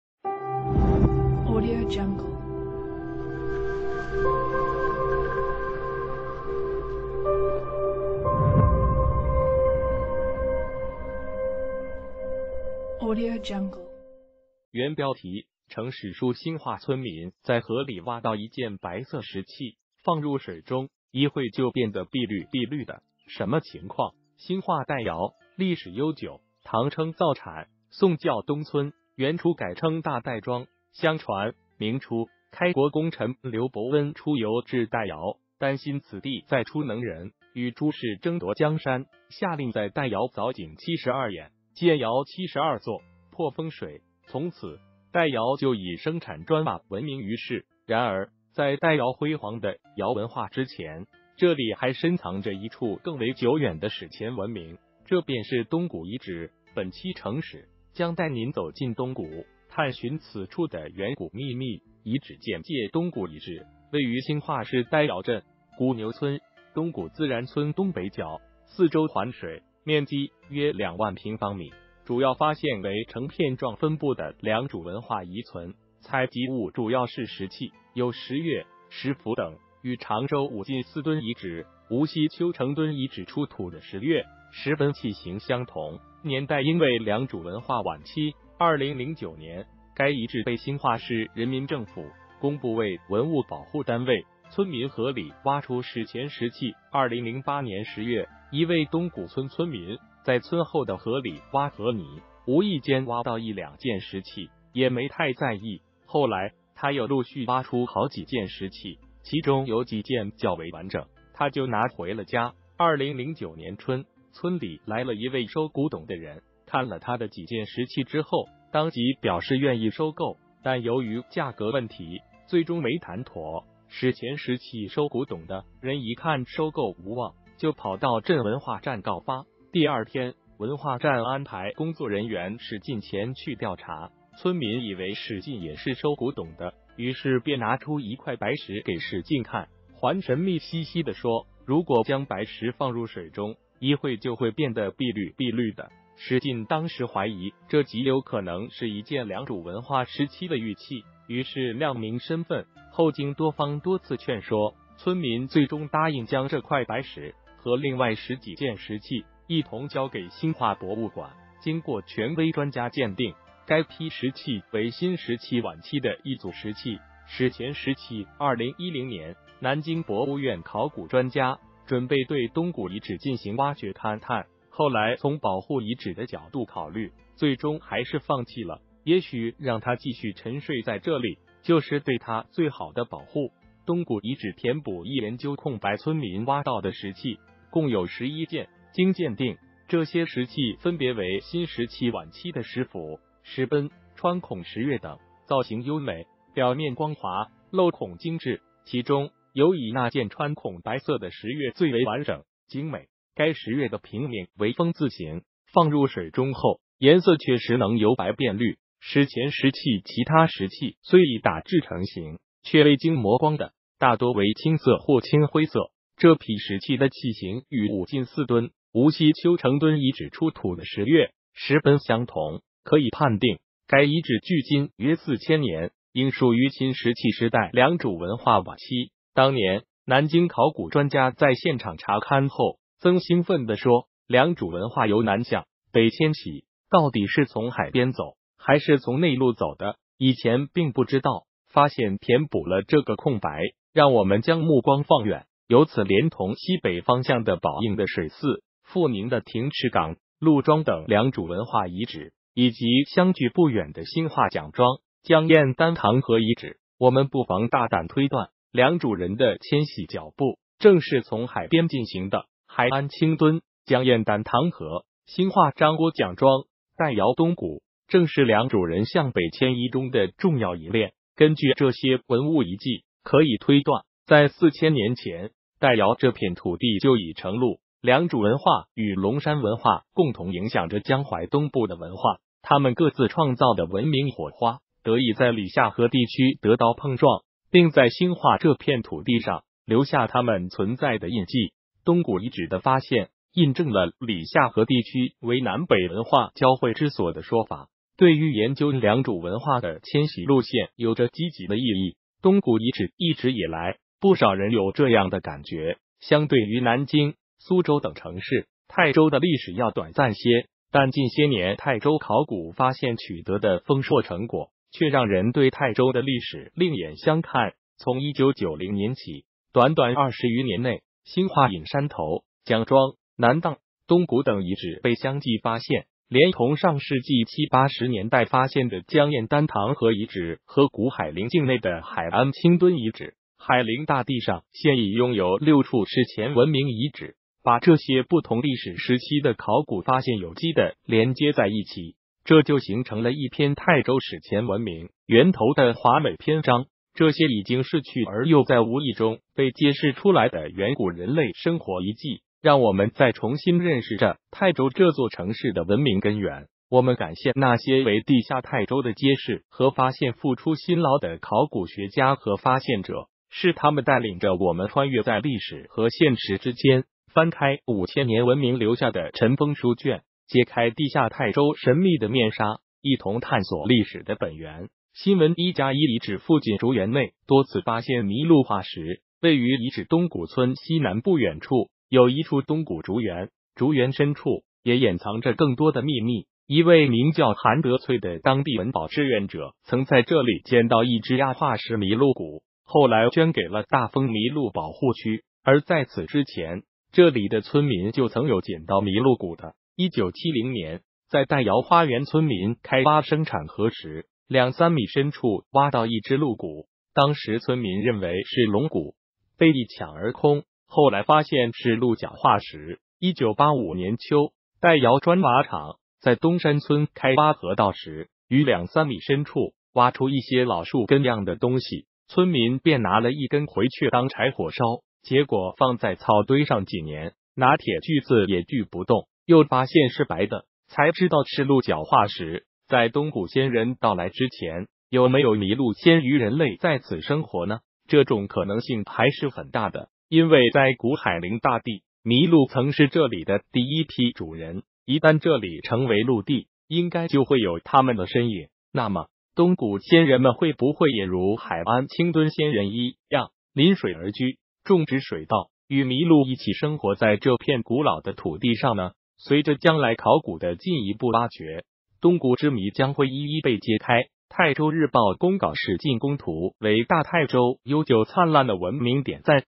城史丨兴化村民在河里挖到一件白色石器，放入水中，一会就变得碧绿碧绿的，什么情况？
原标题：城史丨兴化村民在河里挖到一件白色石器，放入水中，一会就变得碧绿碧绿的，什么情况？兴化戴窑，历史悠久，唐称“灶产”，宋叫“东村”，元初改称“大戴庄”。相传，明初，开国功臣刘伯温出游至戴窑，担心此地再出能人与朱氏争夺江山，下令在戴窑凿井72眼，建窑72座，破风水。从此，戴窑就以生产砖瓦闻名于世。然而，在戴窑辉煌的窑文化之前，这里还深藏着一处更为久远的史前文明，这便是“东古遗址”。本期城史，将带您走进“东古”，探寻此处的远古秘密……【遗址简介】东古遗址位于兴化市戴窑镇古牛村东古自然村东北角，四周环水，面积约2万平方米。主要发现为呈片状分布的良渚文化遗存，采集物主要是石器，有石钺、石斧等，与常州武进寺墩遗址、无锡邱承墩遗址出土的石钺、石锛器形相同，年代应为良渚文化晚期。2009年，该遗址被兴化市人民政府公布为文物保护单位。村民河里挖出史前石器2008年10月，一位东古村村民在村后的河里挖河泥，无意间挖到一两件石器，也没太在意。后来，他又陆续挖出好几件石器，其中有几件较为完整，他就拿回了家。2009年春，村里来了一位收古董的人，看了他的几件石器之后，当即表示愿意收购，但由于价格问题，最终没谈妥。史前石器收古董的人一看收购无望，就跑到镇文化站告发。第二天，文化站安排工作人员史进前去调查。村民以为史进也是收古董的，于是便拿出一块白石给史进看，还神秘兮兮地说，如果将白石放入水中，一会就会变得碧绿碧绿的。史进当时怀疑这极有可能是一件良渚文化时期的玉器，于是亮明身份。后经多方多次劝说，村民最终答应将这块白石和另外十几件石器一同交给兴化博物馆。经过权威专家鉴定，该批石器为新石器晚期的一组石器。史前石器2010年，南京博物院考古专家准备对东古遗址进行挖掘勘探，后来从保护遗址的角度考虑，最终还是放弃了。也许让它继续沉睡在这里，就是对它最好的保护。东古遗址填补一研究空白村民挖到的石器，共有11件。经鉴定，这些石器分别为新石器晚期的石斧、石锛、穿孔石钺等，造型优美，表面光滑，镂孔精致。其中，尤以那件穿孔白色的石钺最为完整、精美。该石钺的平面为“风”字形，放入水中后，颜色确实能“由白变绿”。史前石器其他石器虽已打制成形，却未经磨光的，大多为青色或青灰色。这批石器的器形与武进寺墩、无锡邱承墩遗址出土的石钺、石锛相同。可以判定，该遗址距今约4000年，应属于新石器时代良渚文化晚期。当年，南京考古专家在现场查勘后，曾兴奋地说：“良渚文化由南向北迁徙，到底是从海边走，还是从内陆走的，以前并不知道，发现填补了这个空白。”让我们将目光放远，由此连同西北方向的宝应的水泗、阜宁的停翅港、陆庄等良渚文化遗址，以及相距不远的兴化蒋庄、姜堰单塘河遗址，我们不妨大胆推断，良渚人的迁徙脚步，正是从海边进行的，海安青墩、姜堰单塘河、兴化张郭蒋庄、戴窑东古正是良渚人向北迁移中的重要一链。根据这些文物遗迹，可以推断，在4000年前，戴窑这片土地就已成陆。良渚文化与龙山文化共同影响着江淮东部的文化，它们各自创造的文明火花得以在里下河地区得到碰撞，并在兴化这片土地上留下他们存在的印记。东古遗址的发现，印证了里下河地区为南北文化交汇之所的说法，对于研究良渚文化的迁徙路线有着积极的意义。东古遗址一直以来，不少人有这样的感觉：相对于南京、苏州等城市，泰州的历史要短暂些。但近些年泰州考古发现取得的丰硕成果，却让人对泰州的历史另眼相看：从1990年起，短短20余年内，兴化影山头、蒋庄、南荡、东古等遗址被相继发现。连同上世纪七八十年代发现的姜堰单塘河遗址和古海陵境内的海安青墩遗址，海陵大地上现已拥有6处史前文明遗址。把这些不同历史时期的考古发现有机地连接在一起，这就形成了一篇泰州史前文明源头的华美篇章。这些已经逝去而又在无意中被揭示出来的远古人类生活遗迹，让我们在重新认识着泰州这座城市的文明根源。我们感谢那些为“地下泰州”的揭示和发现付出辛劳的考古学家和发现者，是他们带领着我们穿越在历史和现实之间，翻开五千年文明留下的尘封书卷，揭开“地下泰州”神秘的面纱，一同探索历史的本源。新闻1+1遗址附近竹园内多次发现麋鹿化石位于遗址东古村西南不远处，有一处东古竹园。竹园深处，也掩藏着更多的秘密。一位名叫韩德粹的当地文保志愿者曾在这里捡到一支亚化石麋鹿骨，后来捐给了大丰麋鹿保护区。而在此之前，这里的村民就曾有捡到麋鹿骨的。1970年，在戴窑花元村民开挖生产河时，两三米深处挖到一只鹿骨，当时村民认为是龙骨，被一抢而空，后来发现是鹿角化石。1985年秋，戴窑砖瓦厂在东山村开挖河道时，于两三米深处挖出一些老树根样的东西，村民便拿了一根回去当柴火烧，结果放在草堆上几年，拿铁锯子也锯不动，又发现是白的，才知道是鹿角化石。在东古先人到来之前，有没有麋鹿先于人类在此生活呢？这种可能性还是很大的。因为在古海陵大地，麋鹿曾是这里的第一批主人，一旦这里成为陆地，应该就会有它们的身影。那么，东古先人们会不会也如海安青墩先人一样，临水而居，种植水稻，与麋鹿一起生活在这片古老的土地上呢？随着将来考古的进一步挖掘，东古之谜将会一一被揭开。泰州日报供稿史进供图为大泰州悠久灿烂的文明点赞！